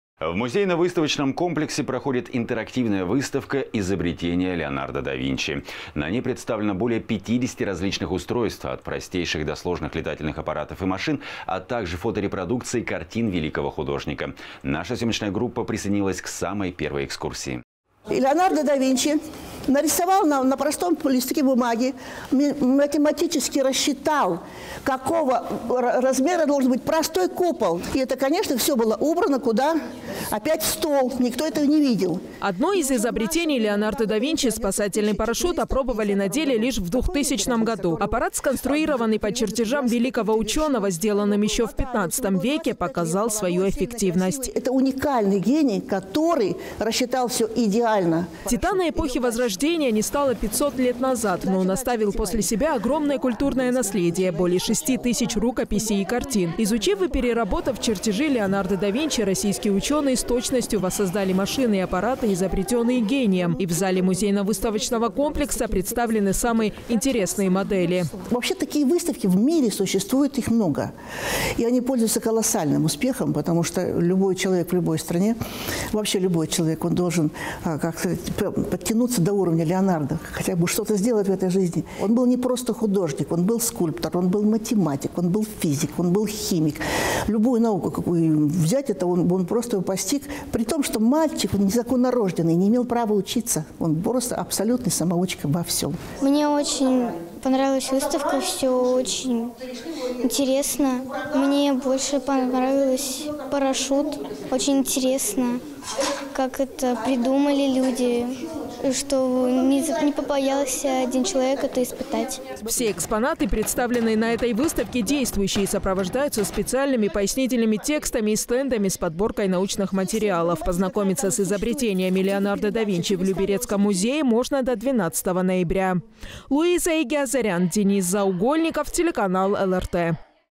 «Изобретения Леонардо да Винчи» в люберецком музее
В Музейно-выставочном комплексе проходит интерактивная выставка «Изобретения Леонардо да Винчи». На ней представлено более 50 различных устройств - от простейших до сложных летательных аппаратов и машин, а также фоторепродукции картин великого художника. Наша съемочная группа присоединилась к первой экскурсии.

«Леонардо да Винчи нарисовал на простом листке бумаги, математически рассчитал, какого размера должен быть простой купол. И это, конечно, все было убрано в стол, этого никто не видел».

Одно из изобретений Леонардо да Винчи – спасательный парашют – опробовали на деле лишь в 2000 году. Аппарат, сконструированный по чертежам великого ученого, сделанным еще в 15 веке, показал свою эффективность.
«Это уникальный гений, который рассчитал все идеально».

Титана эпохи Возрождения не стало 500 лет назад, но он оставил после себя огромное культурное наследие – более 6 тысяч рукописей и картин. Изучив и переработав чертежи Леонардо да Винчи, российские ученые с точностью воссоздали машины и аппараты, изобретенные гением. И в зале музейно-выставочного комплекса представлены самые интересные модели.

Наталья Степанова, главный хранитель Люберецкого краеведческого музея: «Вообще такие выставки в мире существуют, их много. И они  пользуются колоссальным успехом, потому что любой человек в любой стране он должен подтянуться до уровня Леонардо хотя бы что-то в этой жизни. Он был не просто художник, он был скульптор, он был математик, он был физик, он был химик. Любую науку он постиг, при том, что мальчик незаконнорождённый, не имел право учиться, он просто абсолютный самоучка во всем».

Маргарита Учайкина, посетитель Музейно-выставочного комплекса г.о. Люберцы: «Мне очень понравилась выставка, все очень интересно. Мне понравился парашют, очень интересно, как это придумали люди и что не побоялся один человек это испытать».

Все экспонаты, представленные на этой выставке, действующие и сопровождаются специальными пояснительными текстами и стендами с подборкой научных материалов. Познакомиться с изобретениями Леонардо да Винчи в люберецком музее можно до 12 ноября.